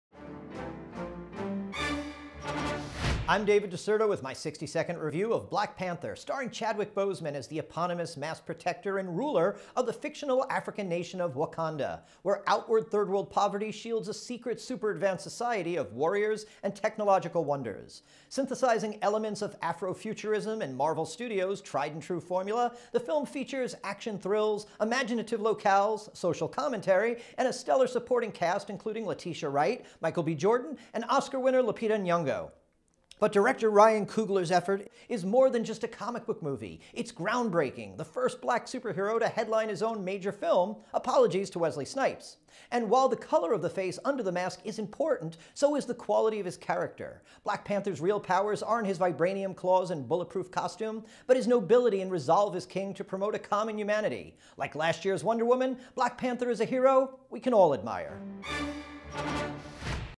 REEL FAITH 60 Second Review of BLACK PANTHER
David DiCerto reviews the latest Marvel Cinematic Universe chapter and  ground-breaking solo adventure of T'Challa in BLACK PANTHER!